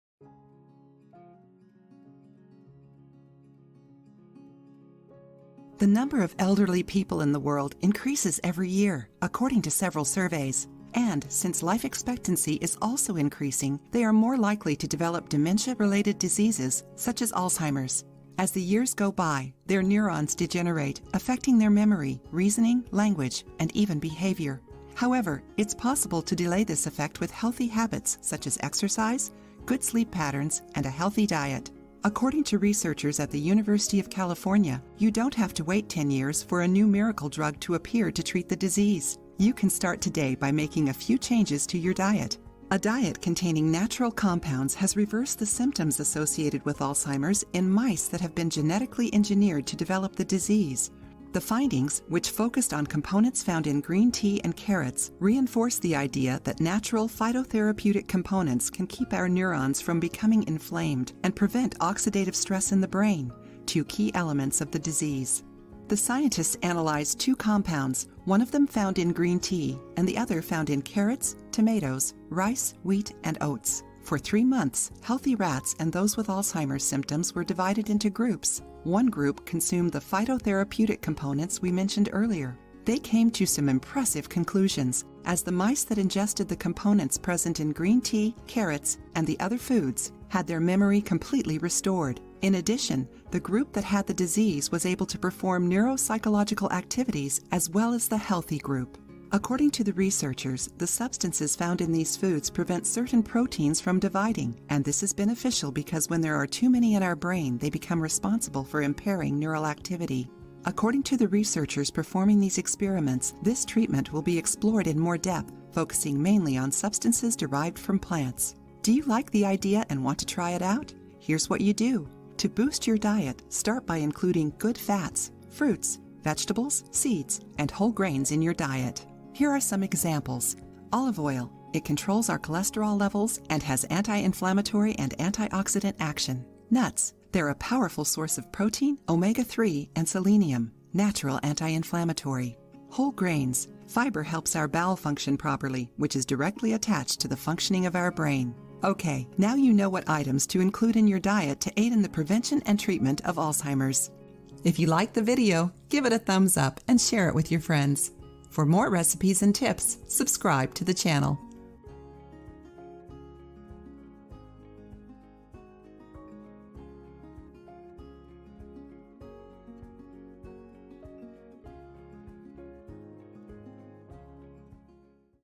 Alzheimer's Symptoms Could Be Reversed By Changing Your Diet, Study Finds
The number of elderly people in the world increases every year, according to several surveys.

And, since life expectancy is also increasing, they are more likely to develop dementia related diseases, such as Alzheimer's.

As the years go by, their neurons degenerate, affecting their memory, reasoning, language and even behavior.

However, it’s possible to delay this effect with healthy habits such as exercise, good sleep patterns, and a healthy diet.

According to researchers at the University of California, you don't have to wait 10 years for a new miracle drug to appear to treat the disease.

You can start today by making a few changes to your diet.

A diet containing natural compounds has reversed the symptoms associated with Alzheimer's in mice that have been genetically engineered to develop the disease.

According to they researchers performing these experiments, this treatment will be explored in more depth focusing mainly on substances derived from plants.

Did you like the idea and want to try it out?

Here’s what you do:

To boost your diet, start by including good fats, fruits, vegetables, seeds, and whole grains in your diet.

Ok, now you know what items to put include in your diet to aid in the prevention and treatment of Alzheimer's.